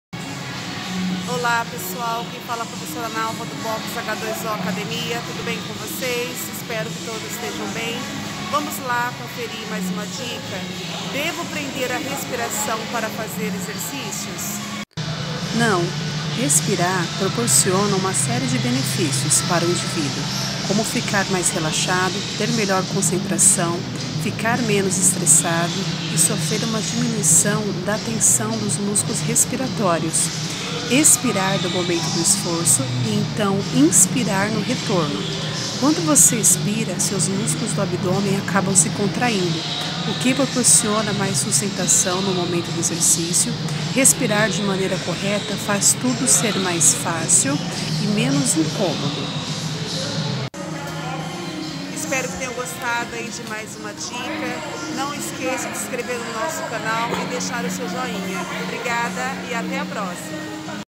Devo prender a respiração quando faço um exercício?
Não, deve respirar de maneira que qdo faz força inspire e qdo vota expire. Confira as informações da prof. Edinalva.